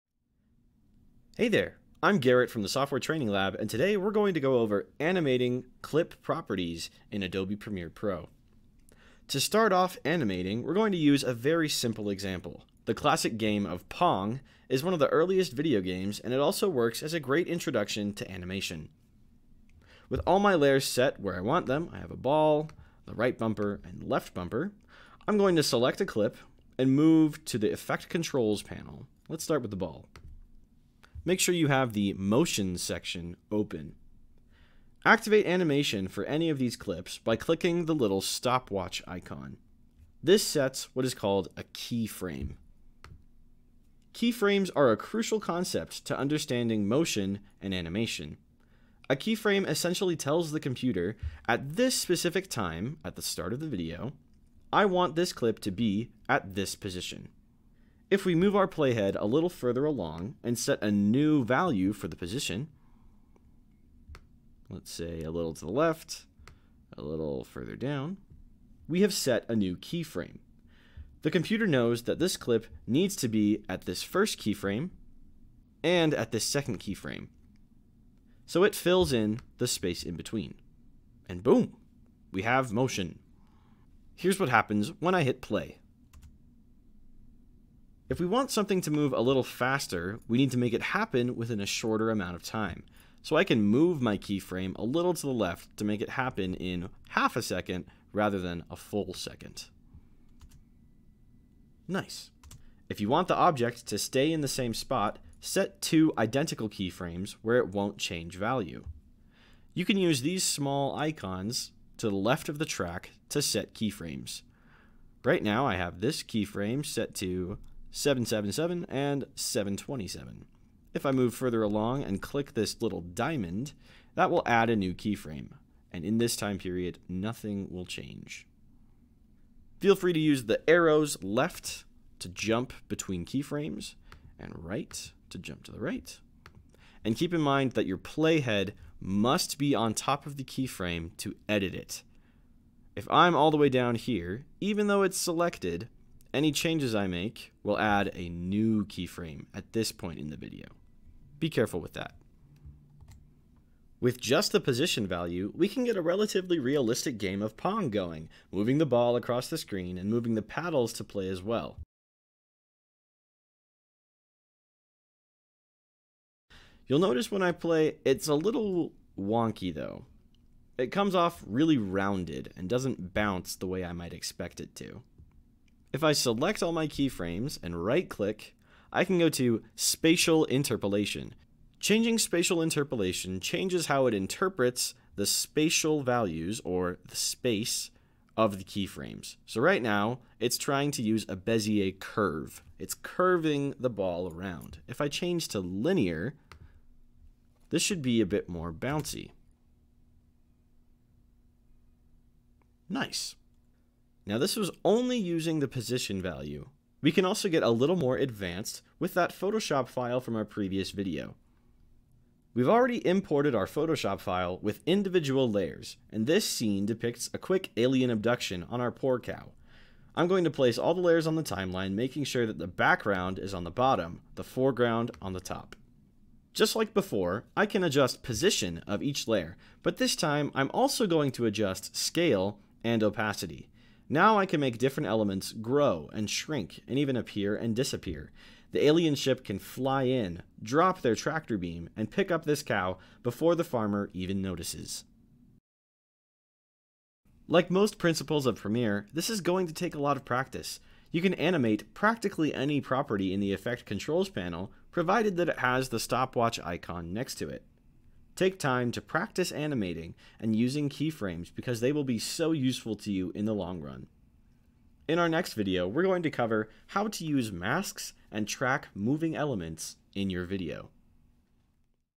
Day 3 - Animating Clip Properties in Adobe Premiere Pro
Garrett from the Software Training Lab covers the basics of animating properites of a clip in Adobe Premiere Pro. Watch through the other videos on the playlist to get an understanding of the fundamentals of Premiere.